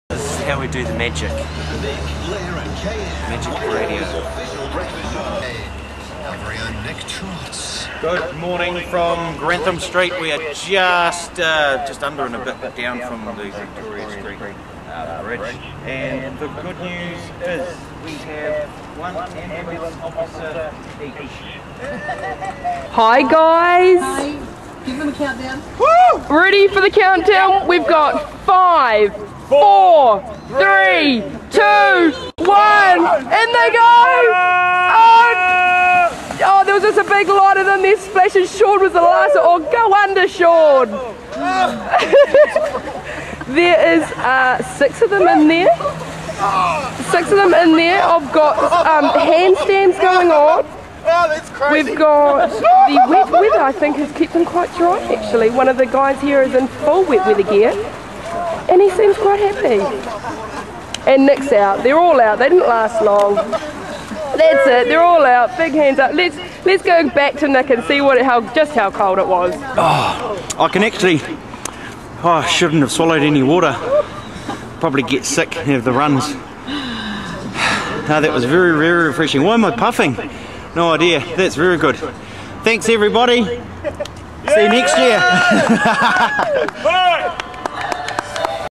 Mid winter swim
92 MORE FM's mid-week mid-winter swim in the Waikato River, Hamilton.